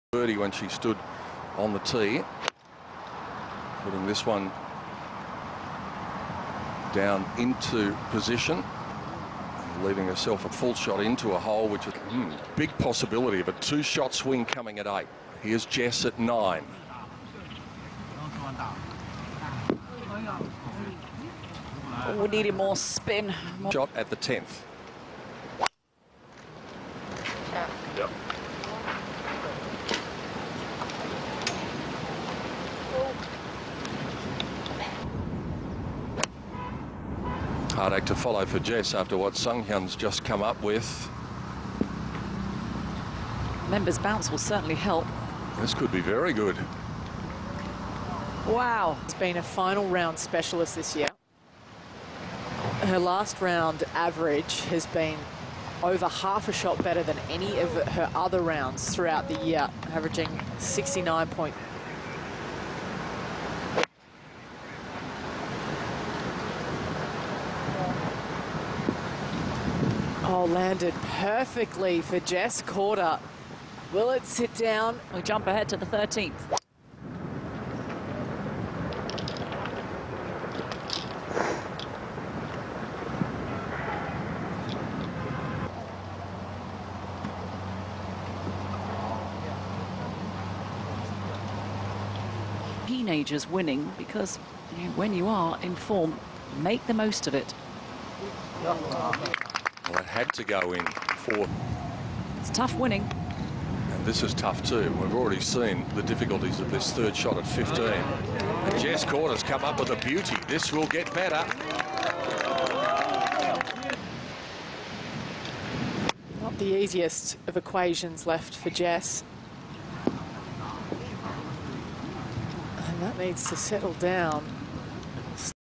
Jessica Korda's Golf Shot Highlights 2017 Blue Bay LPGA Tournament
1 Step To Better Golf |
Golf Best Videos Links Clack Hear And Watch Now